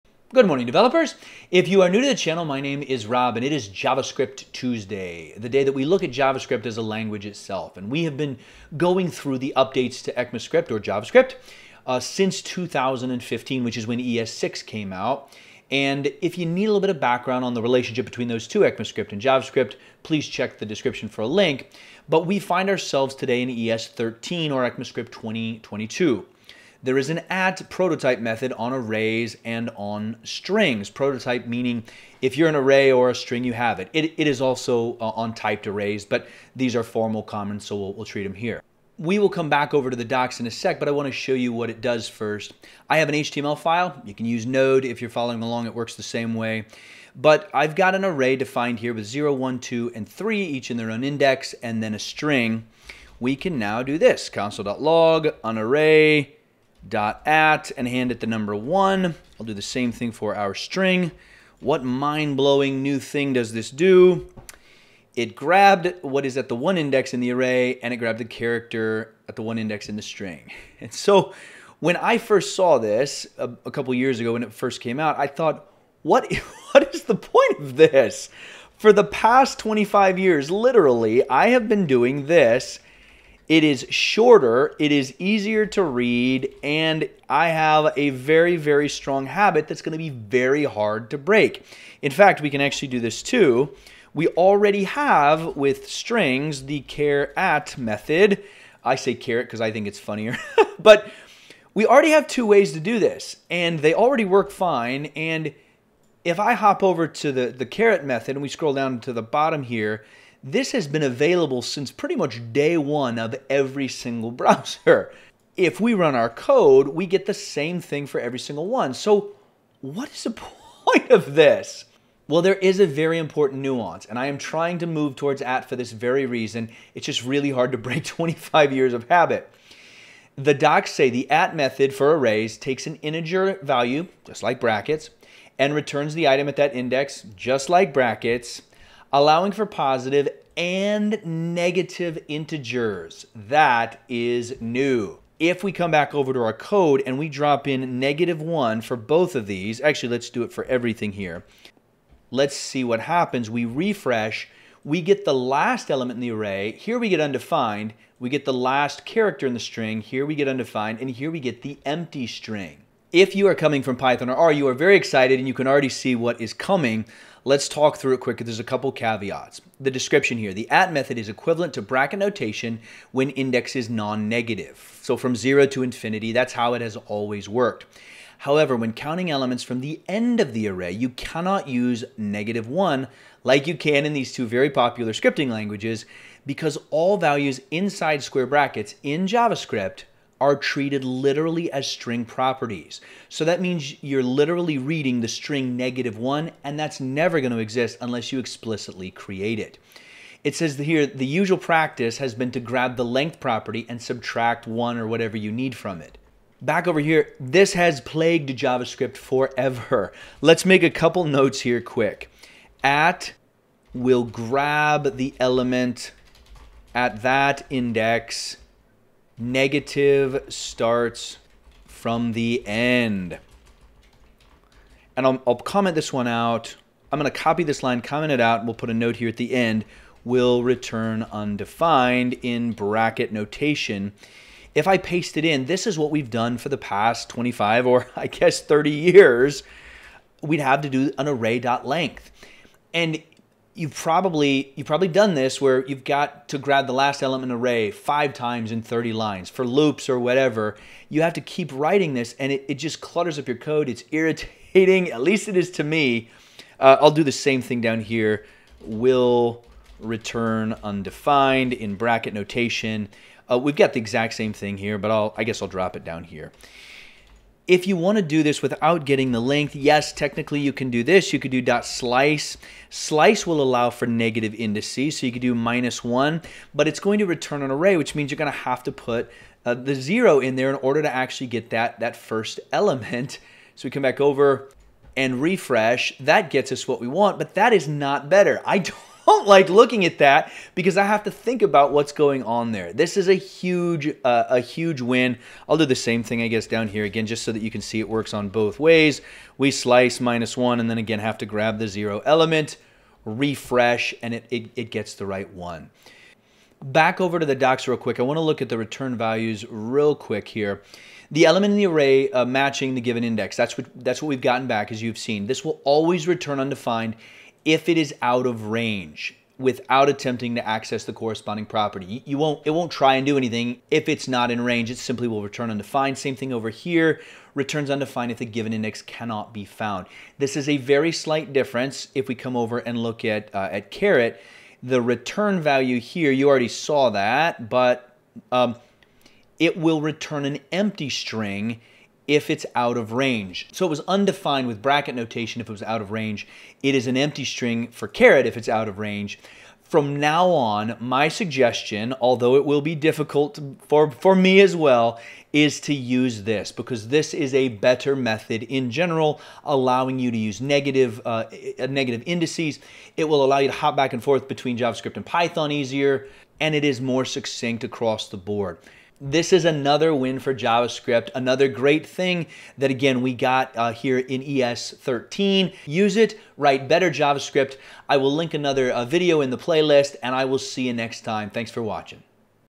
The .at() method - Unleash the Power of JavaScript Updates
There's nothing convenient about changing to it, but a must use feature in ECMAScript 2022 is the .at() method! How is "at" different than charAt? Or how is it different than bracket notation? Why would you want to use it? (If you are coming from Python or R you will be very excited) .Join Rob on Javascript Tuesday as he delves into the advantages of using .at() for arrays and strings over bracket notation, slice, and charAt(). Say goodbye to cumbersome coding with .at() and start writing better JavaScript today!

0:00 - Introduction
0:40 - What is the point of this...
2:00 - The Docs (and the point)
3:30 - Negative indices that work!
4:00 - Goodbye .length - 1
5:50 - Use it!